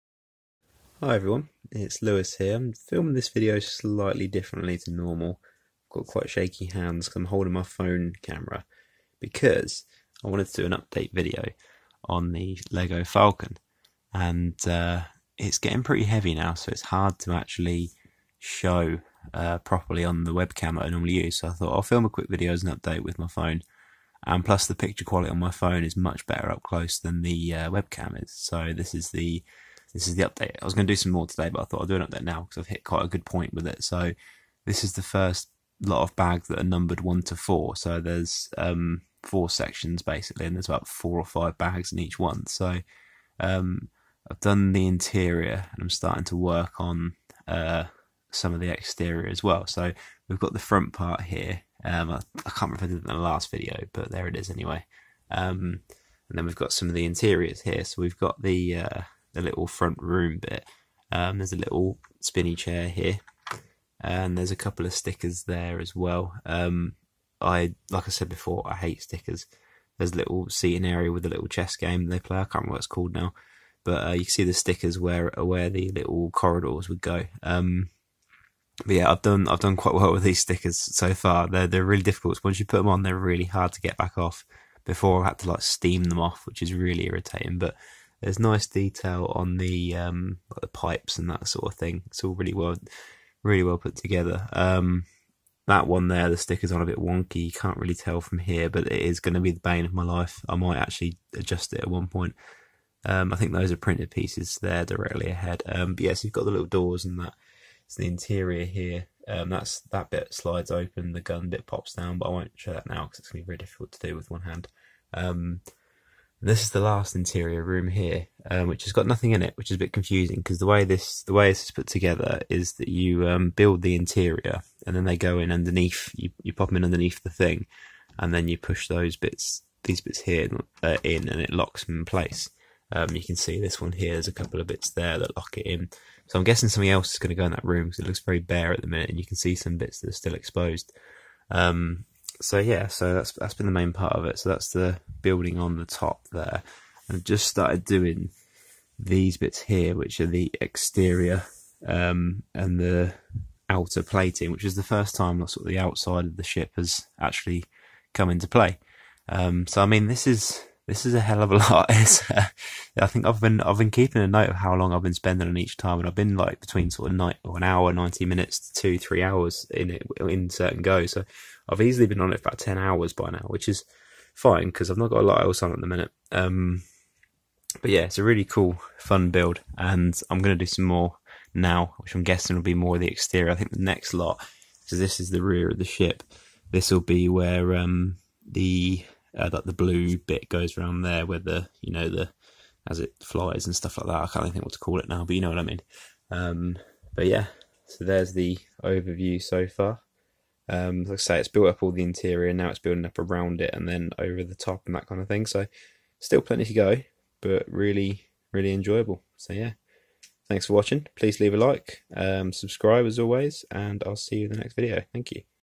LEGO UCS Millennium Falcon update part 2- over 10 hours in, interior complete.
Second update on the Lego UCS Millennium Falcon build, over ten hours in! interior rooms almost complete and starting exterior. getting heavy!